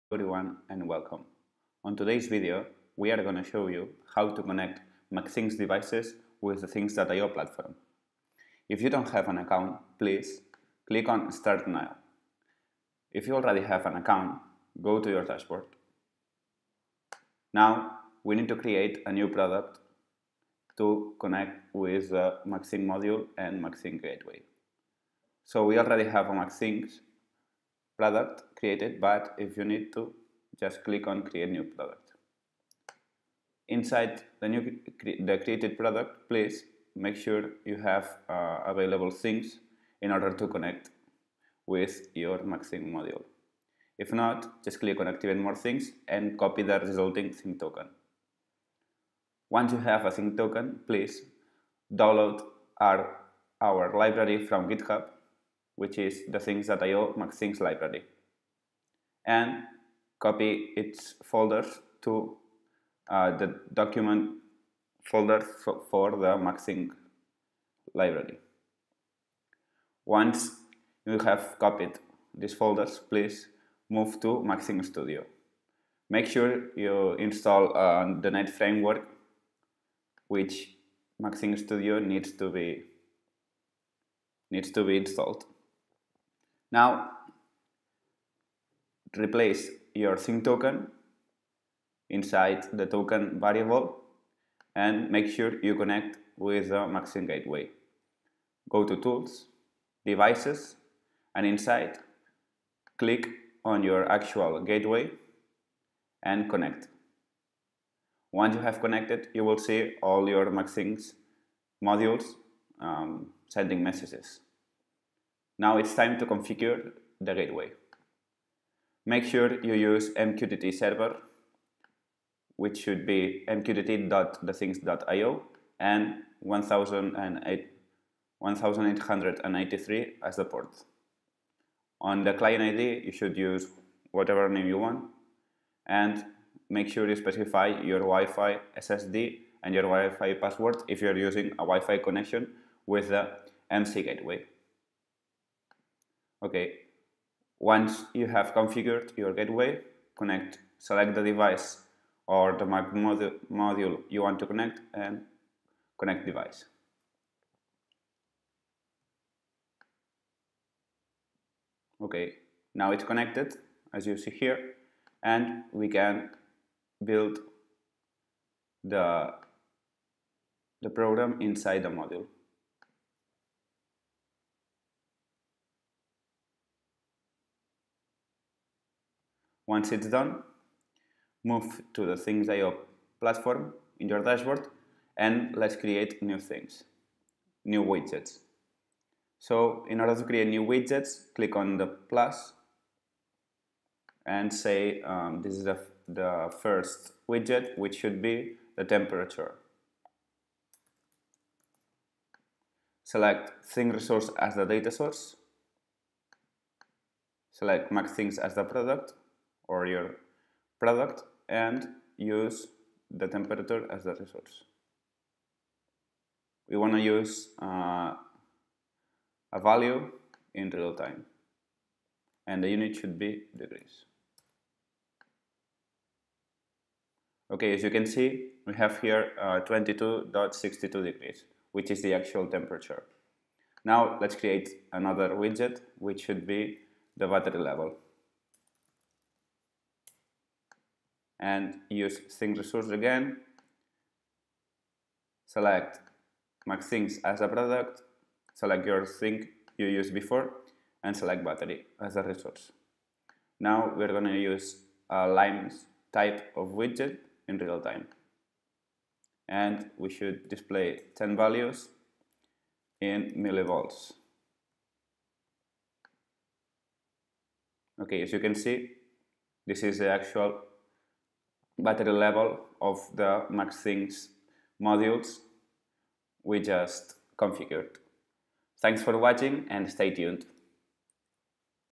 Connecting mcThings with thethings.iO IoT platform
On this video we showcase how to connect mcThings hardware platform with thethings.iO IoT cloud platform.